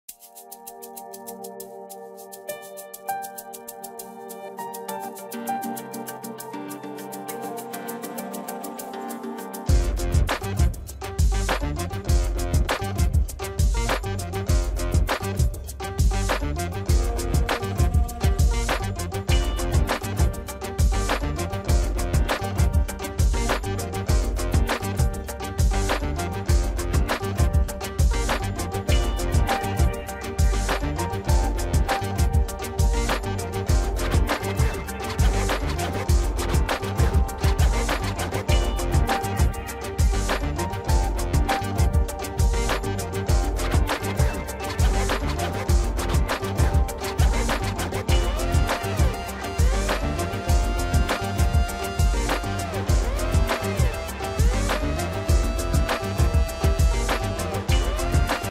Carbon Laser | DermaZeal Clinic | Bangalore @dermazealclinic
With its cutting-edge technology, the Carbon Laser Peel is the latest and most effective laser treatment for addressing a range of skin concerns, including dark spots, hyperpigmentation, sun damage, and skin rejuvenation.